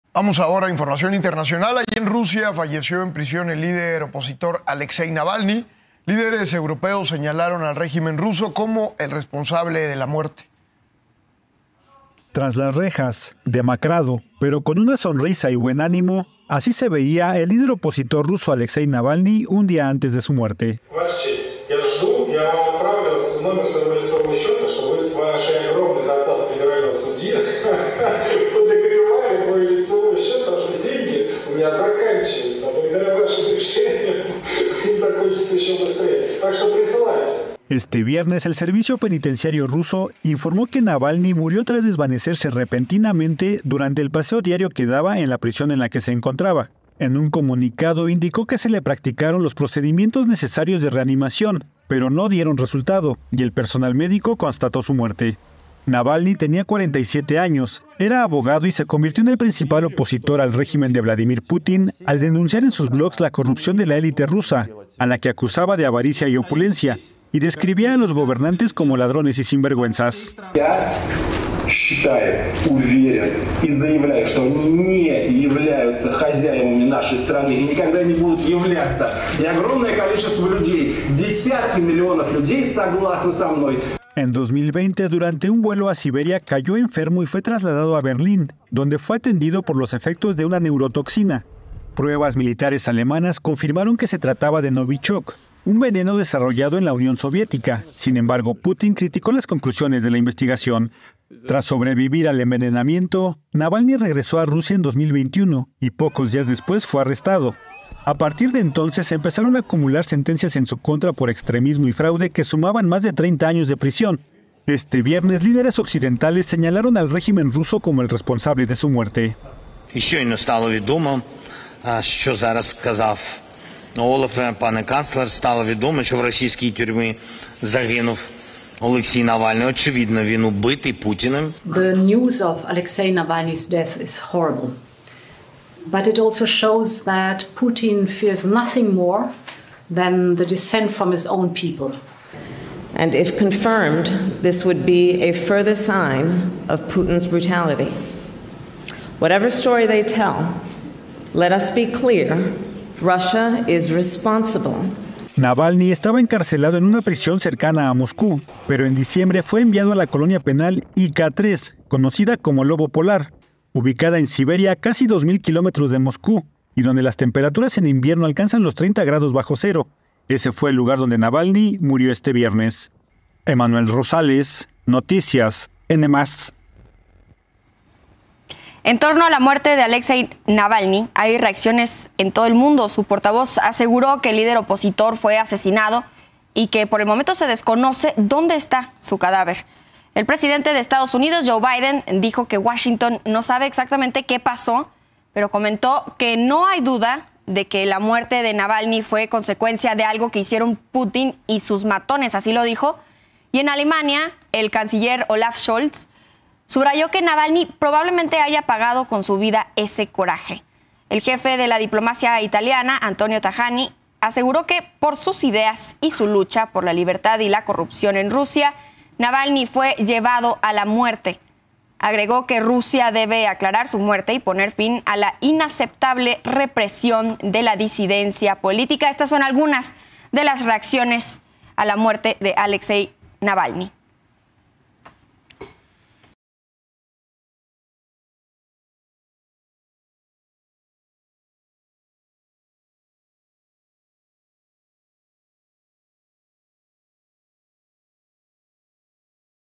Esto se sabe de la muerte de Alexei Navalny, opositor ruso - Las Noticias
Tras el anuncio de la muerte de Alexei Navalny hay reacciones y condenas en el mundo. Su portavoz aseguró que el líder opositor fue asesinado y que por el momento se desconoce dónde está su cadáver.